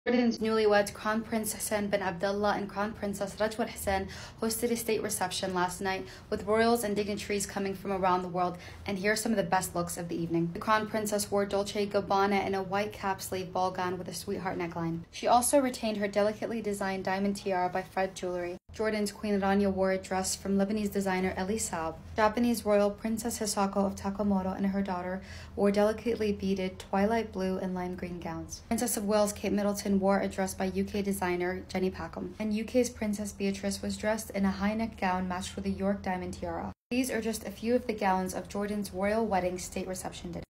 A closer look at some of the royal looks from the Jordanian reception | Arab News
As the royal wedding of PrincessRajwa and CrownPrinceHussein came to a close last night in Jordan, we take a look at some of the royal looks from the glittering state reception.